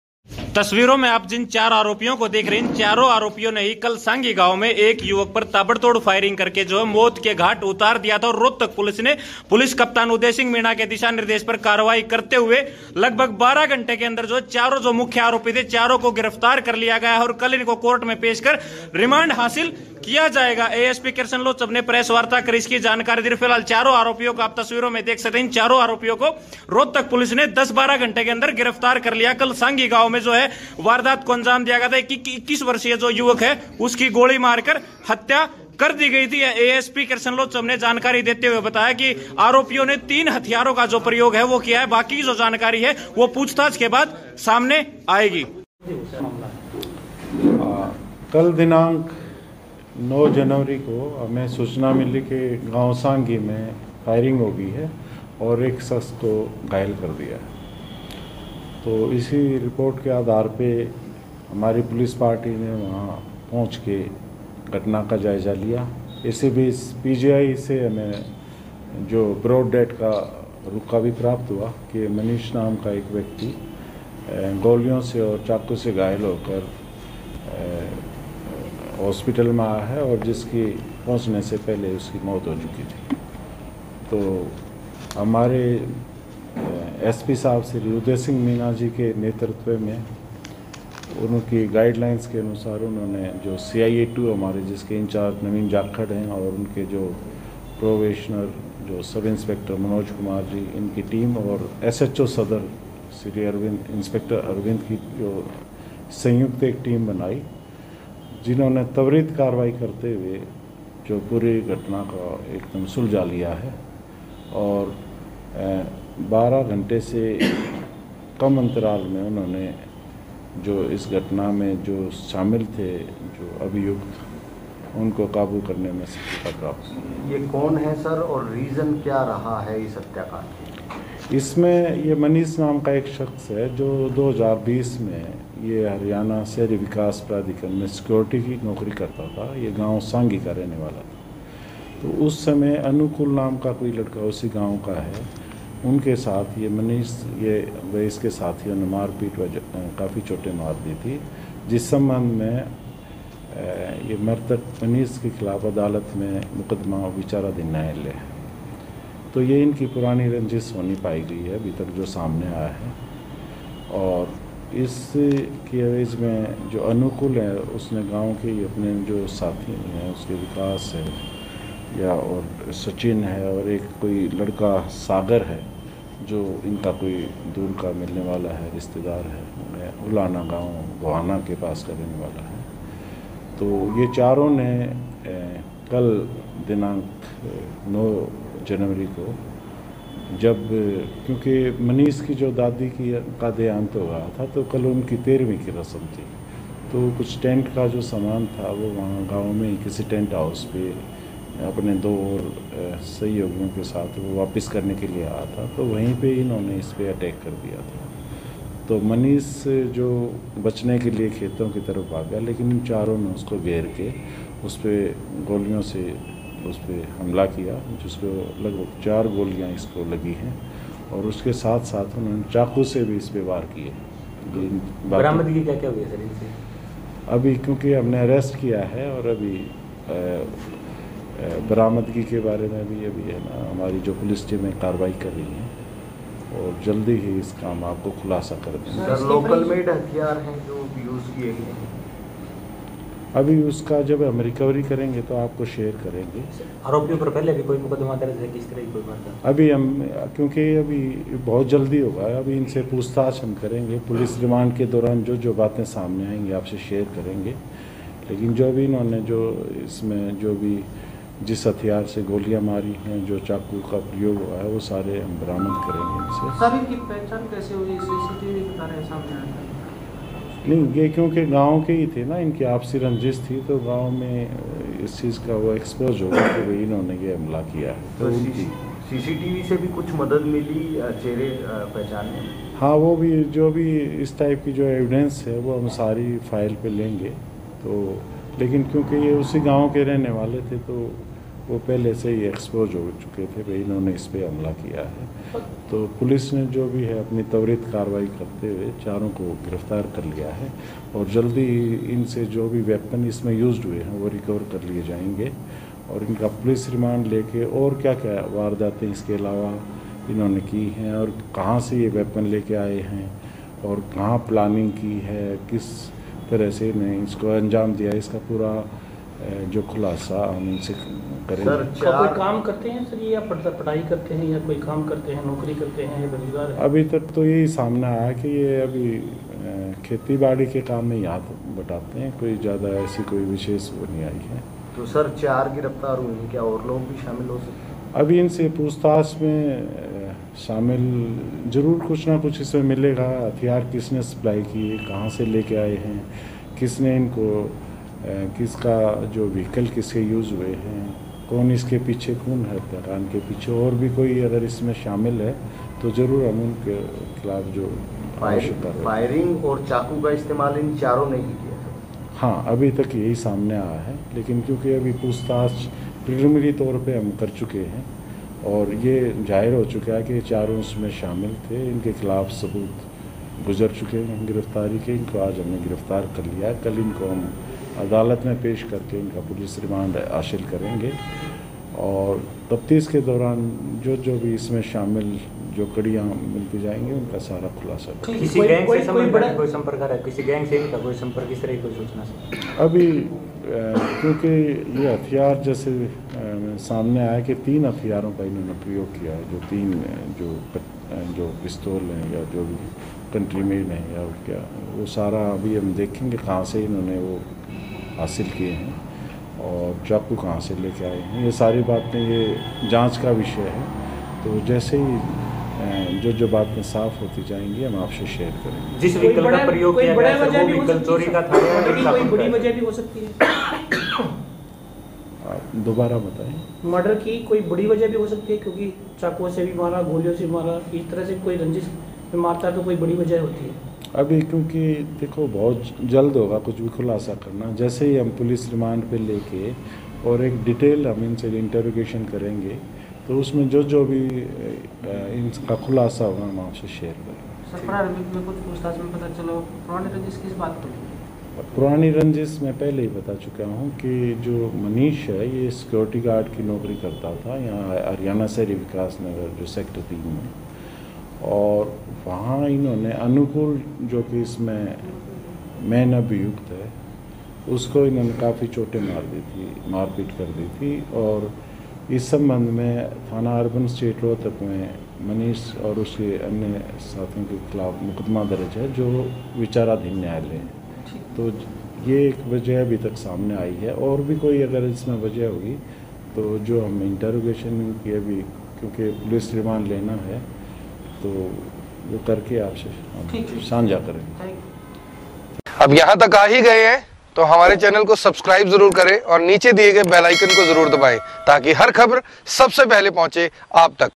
पूर्व मुख्यमंत्री के गांव सांघी में हुए मर्डर मामलें में CIA-2 ने 4 आरोपियों को किया गरफ्तार
पूर्व मुख्यमंत्री के गांव सांघी में हुए मर्डर मामलें में CIA-2 ने 4 आरोपियों को किया गरफ्तार, पुरानी रंजिस के चलते दिया वारदात को अंजाम